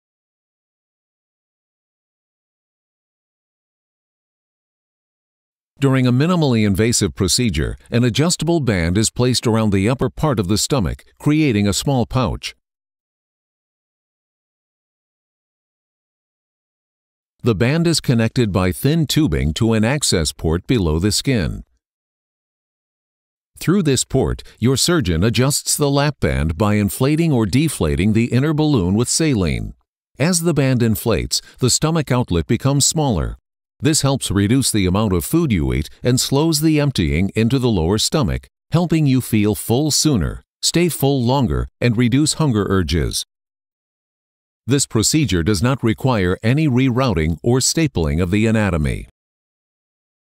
Adjustable Gastric Band, East Cooper Bariatrics
The Adjustable Gastric Band – often called the band – involves an inflatable band that is placed around the upper portion of the stomach, creating a small stomach pouch above the band, and the rest of the stomach below the band.